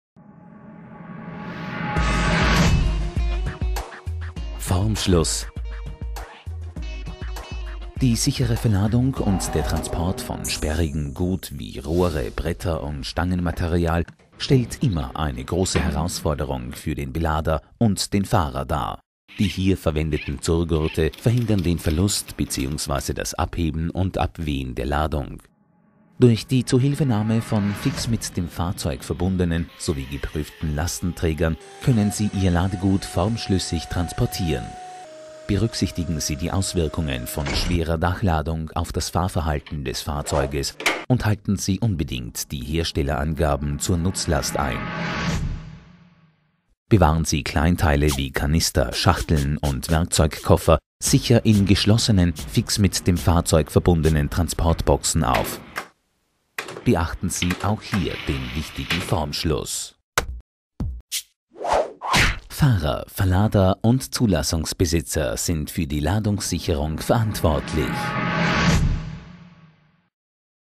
Ladungssicherung auf Pritschenfahrzeugen - Transport von Rohren und Kofferwerkzeugen
Die Ladungssicherung von langen Rohren und Kofferwerkzeugen auf Pritschenfahrzeugen ist enorm wichtig, damit die Ladung nicht abhebt und die Sicherheit im Straßenverkehr gewährleistet wird. Mit Hilfe von fix mit dem Fahrzeug verbundenen Ladungsträgern und Verzurrmitteln kann ein Formschluss der Ladung hergestellt werden.